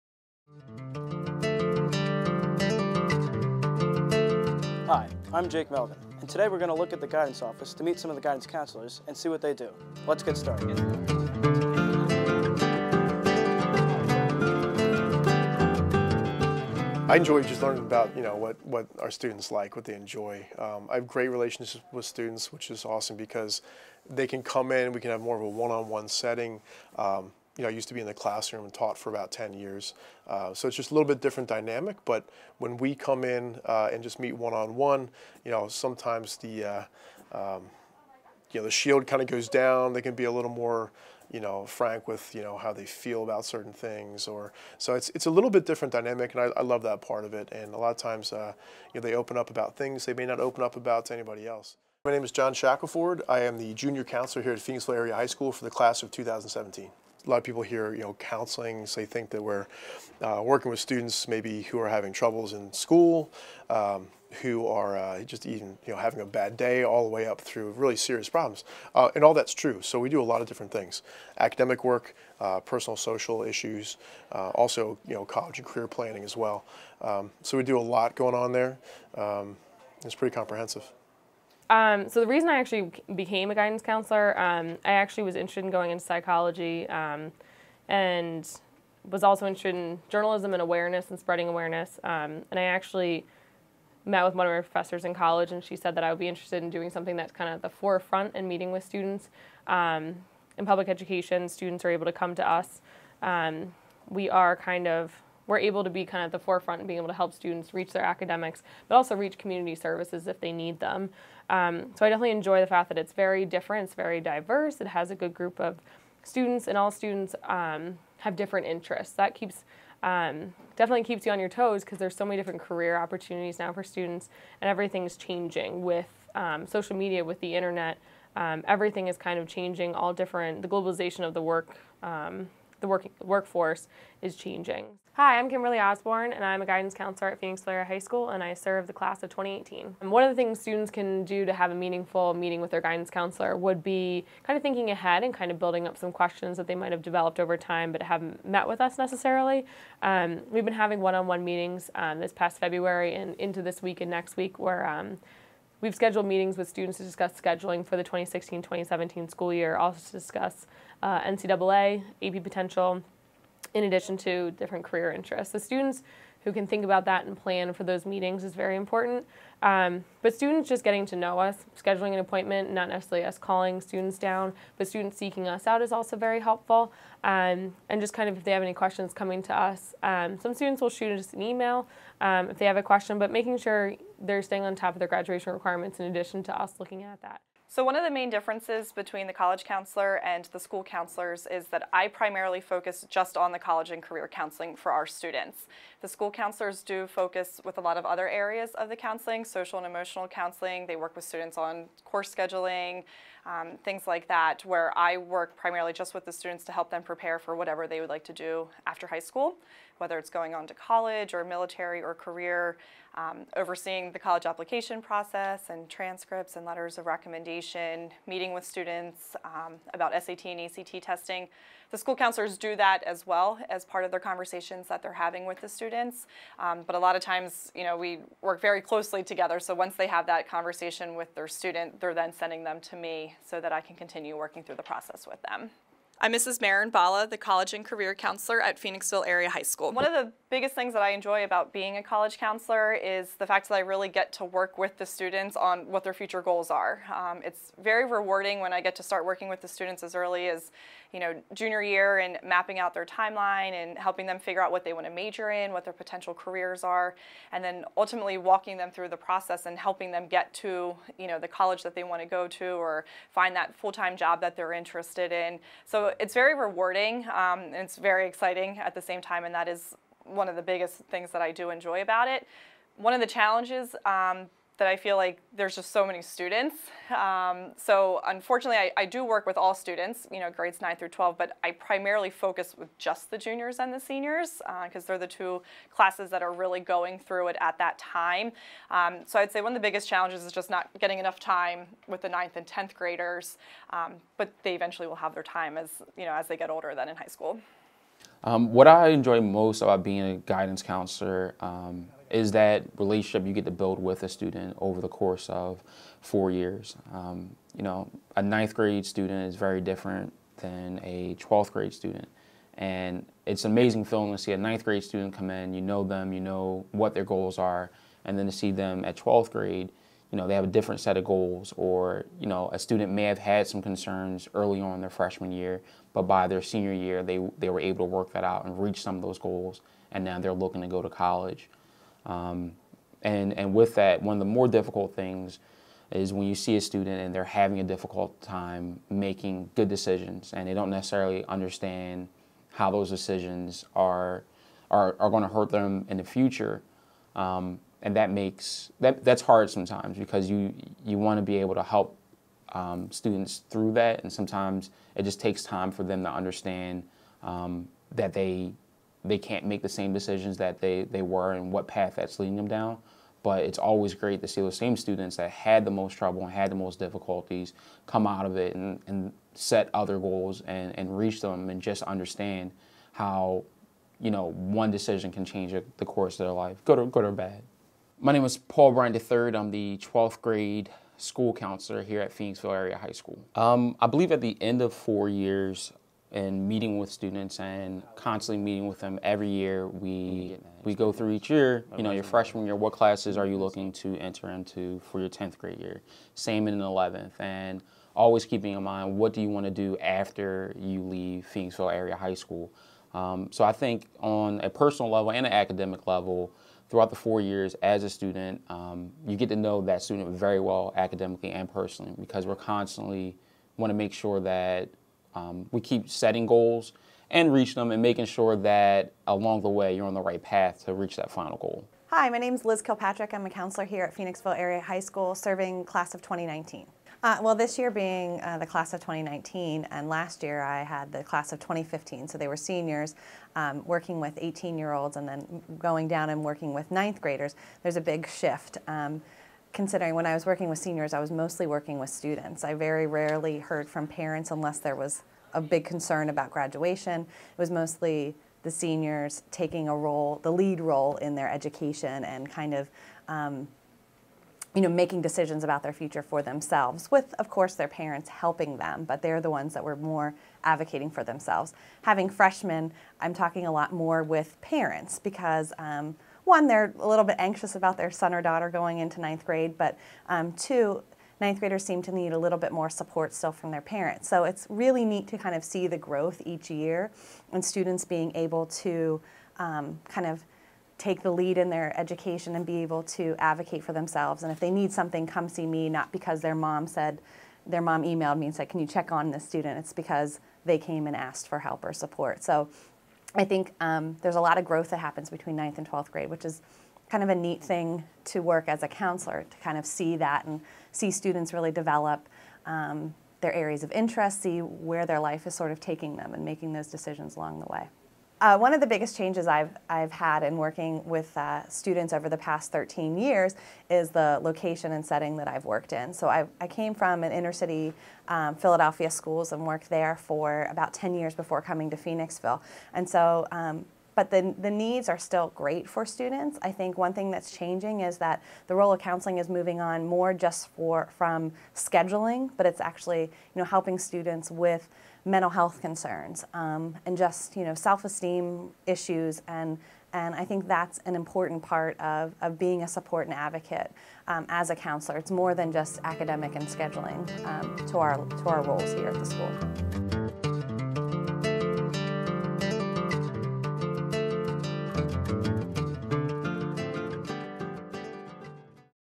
Phantom TV Profile: PAHS Guidance Department
The Period 8 TV Production Class recently visited the Phoenixville Area High School Guidance Department. The class visited each counselor and talked with them on their roles and what they like about being a counselor at the school.